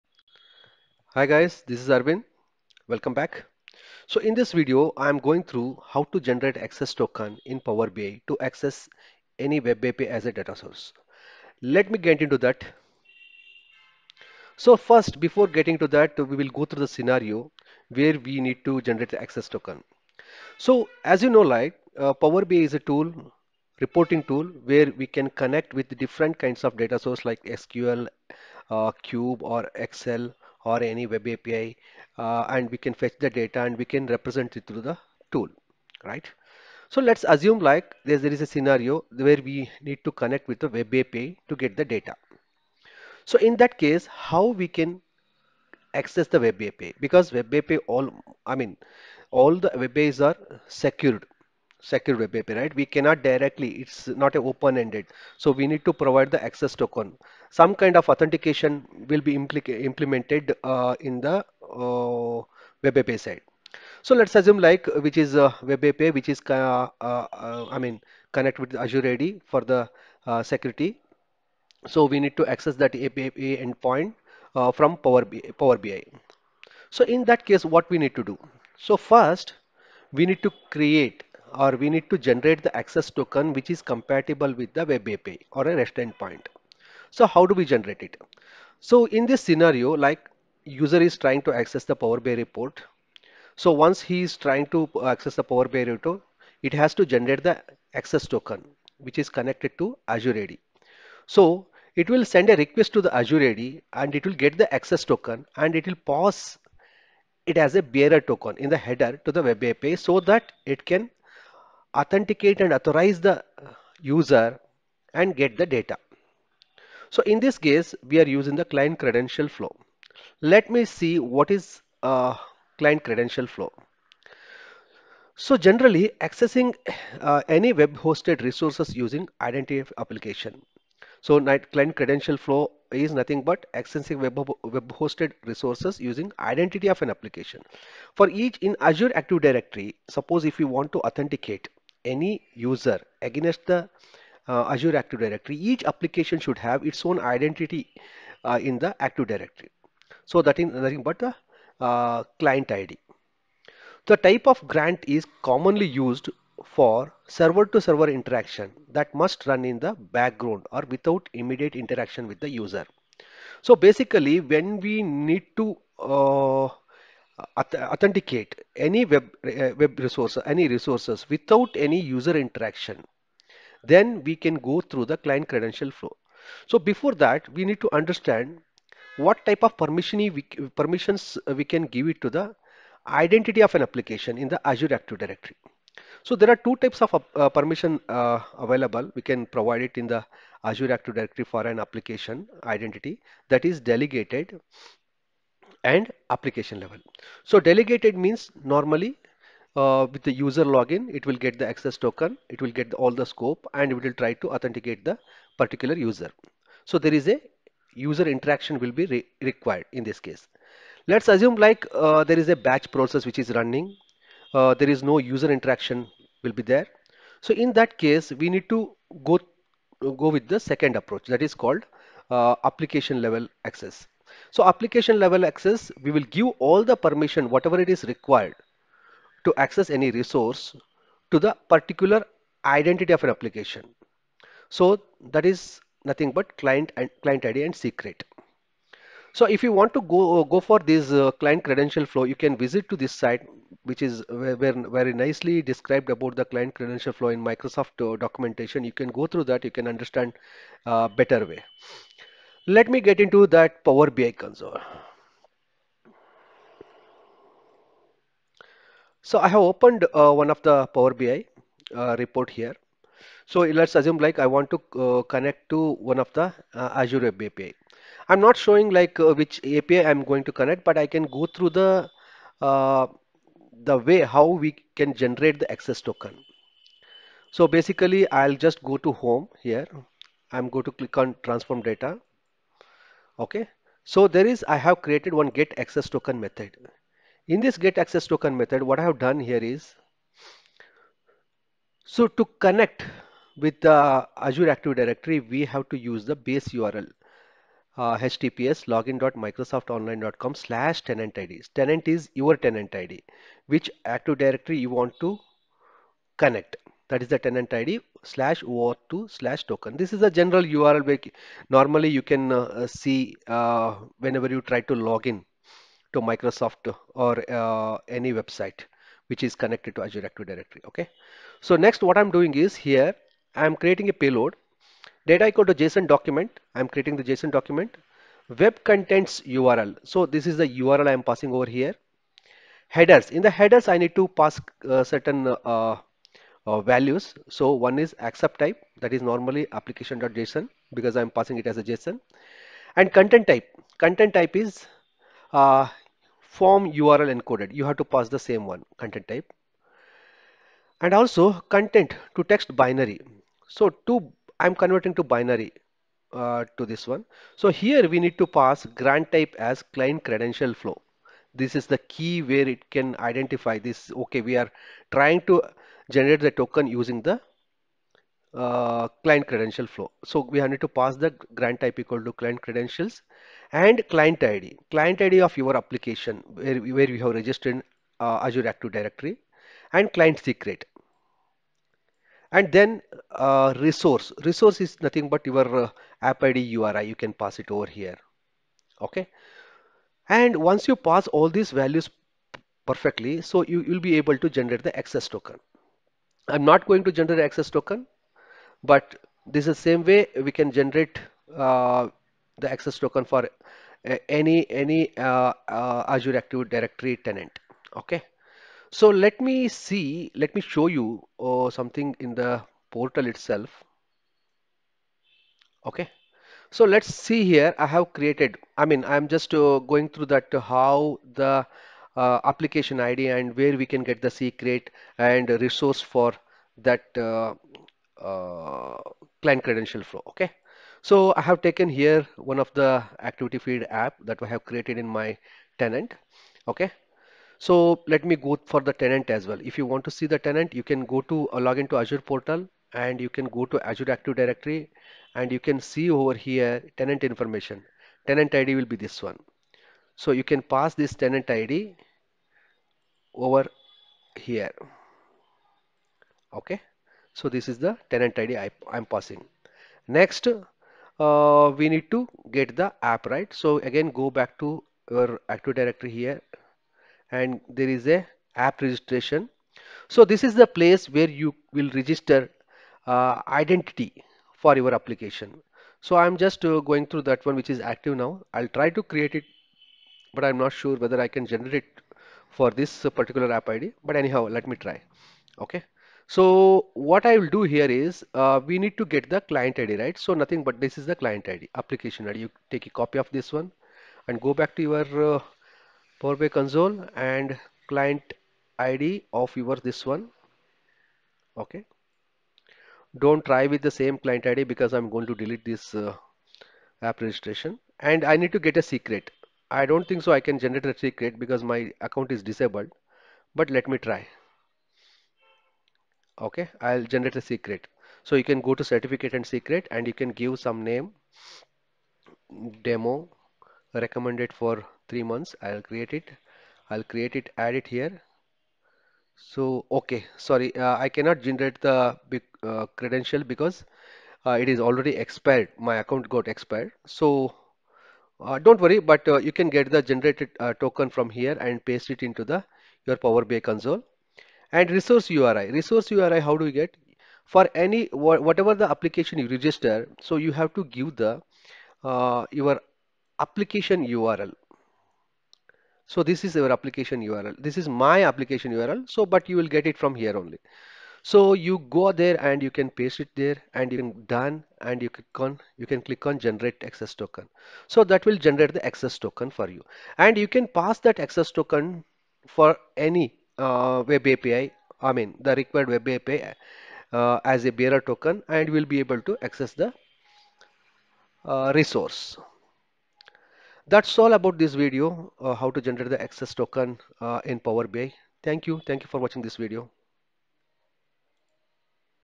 Power BI Access Token Tutorial | Client Credential Flow with Azure AD
Learn how to generate an access token for Power BI using the Client Credential Flow in Azure Active Directory. This tutorial walks you through app registration, permission setup, and token generation for secure API access.
🔹 Step-by-step guide for Azure AD App Registration
🔹 How to use Client Credential Flow for Power BI REST API
🔹 Secure your Power BI integrations with best practices